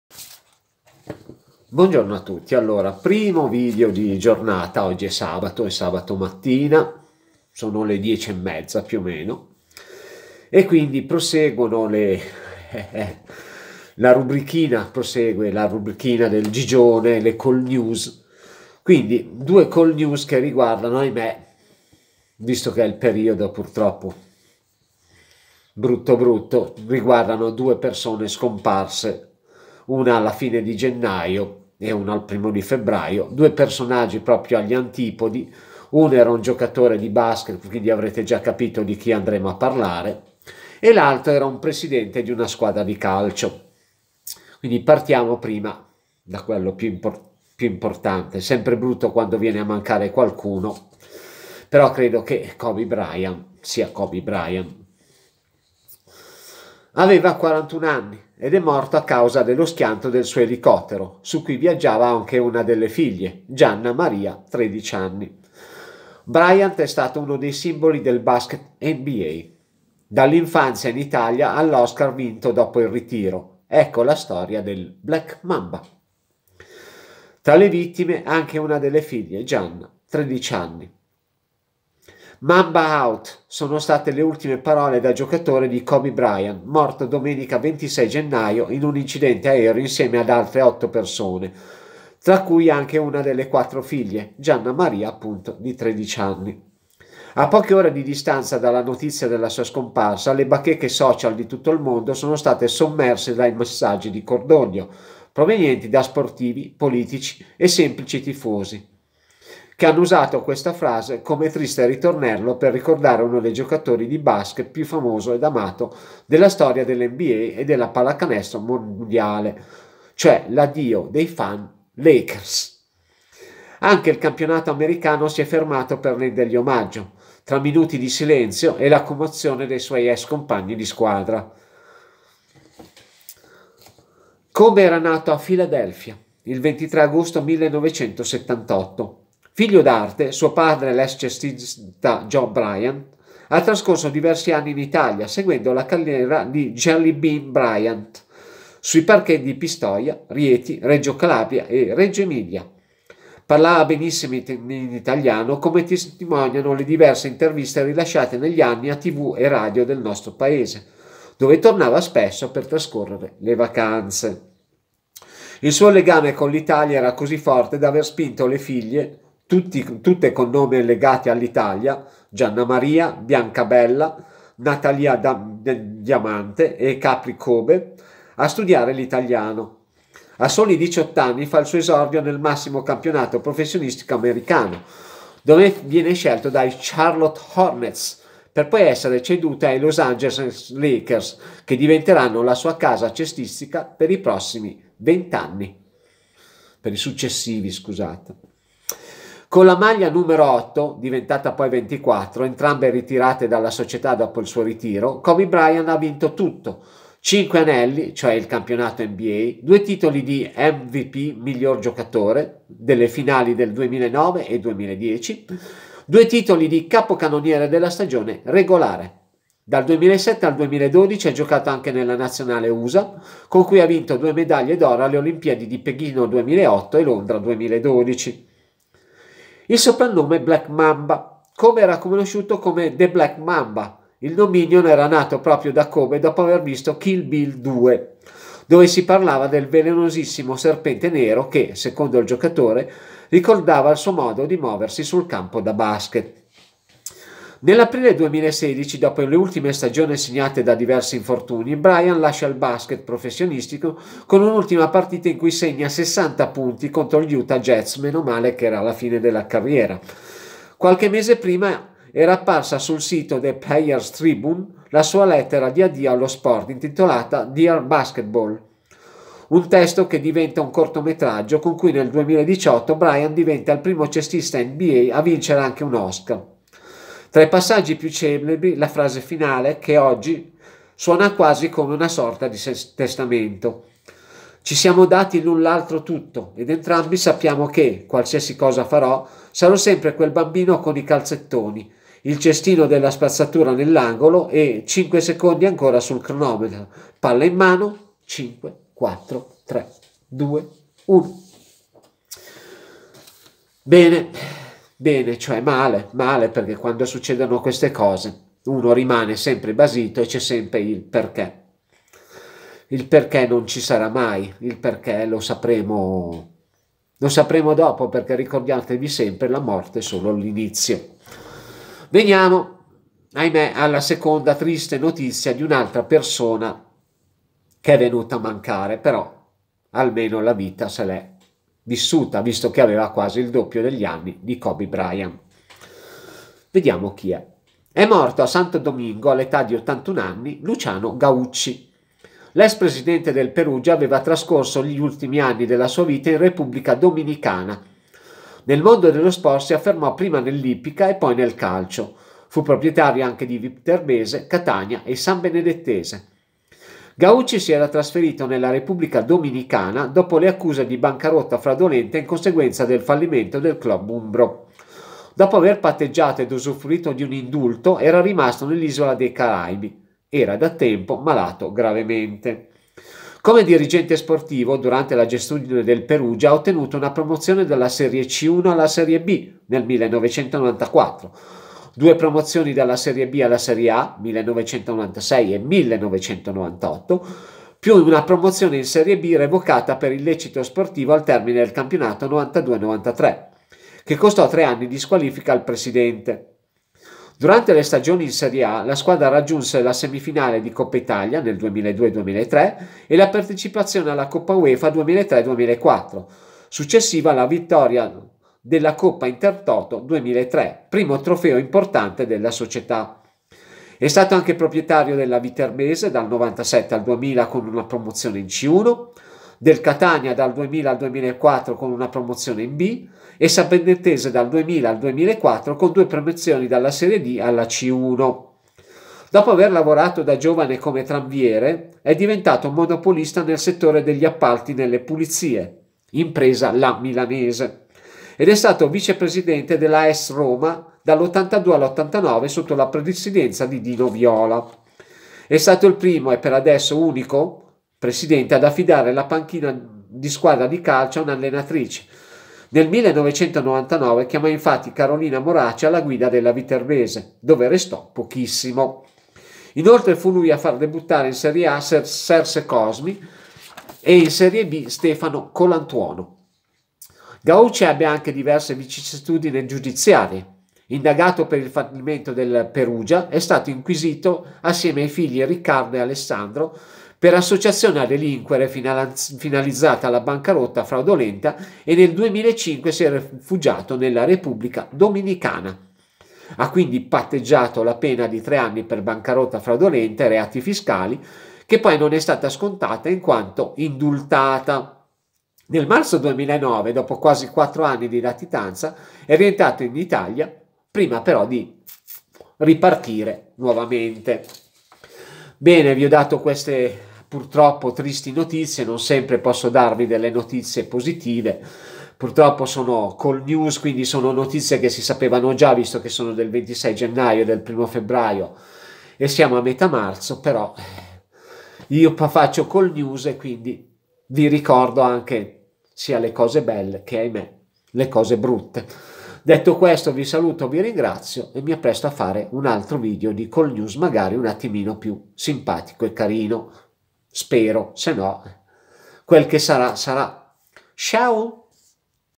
Le notizie del Gigione le cold news, indizi: campione e presidente
Purtroppo il ricordo di due personaggi che sono agli antipodi ma che ci hanno lasciati quasi nello stesso periodo